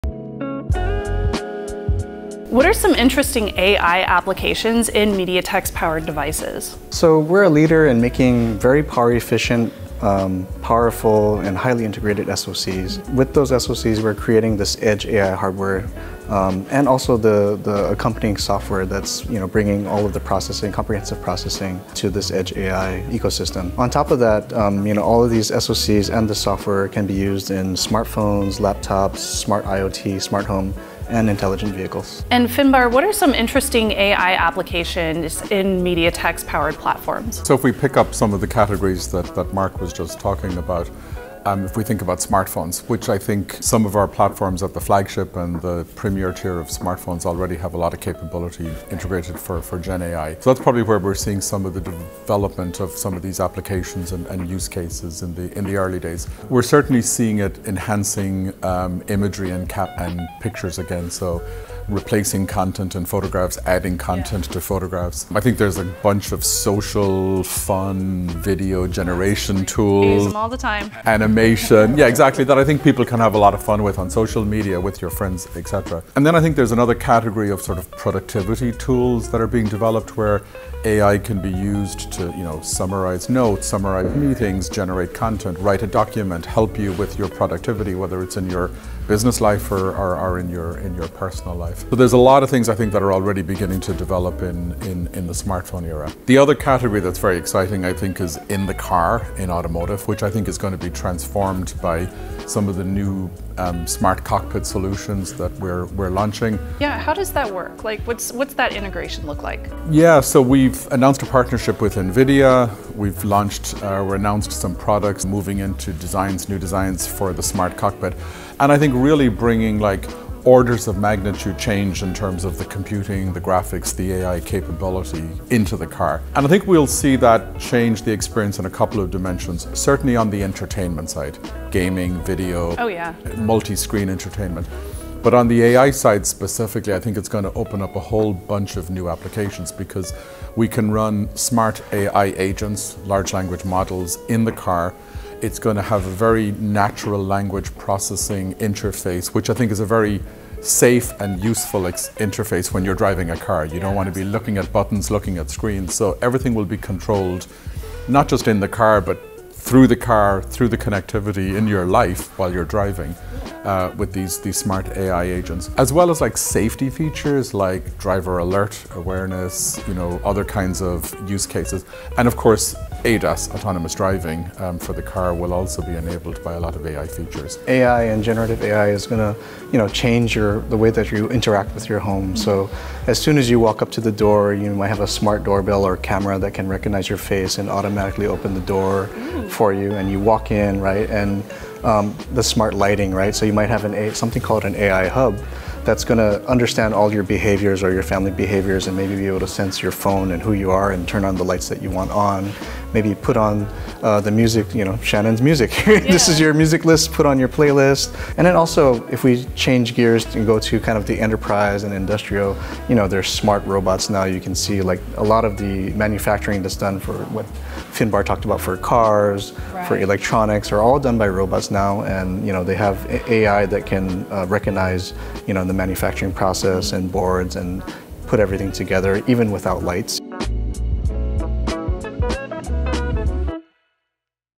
Powering cutting-edge AI applications with MediaTek
From cutting edge smartphones and tablets, to data centers and smart vehicles, MediaTek powers it all. MediaTek’s AI innovation integrated into its SoCs not only make devices smarter and more personalized, but they also drive better human outcomes, productivity, and creativity.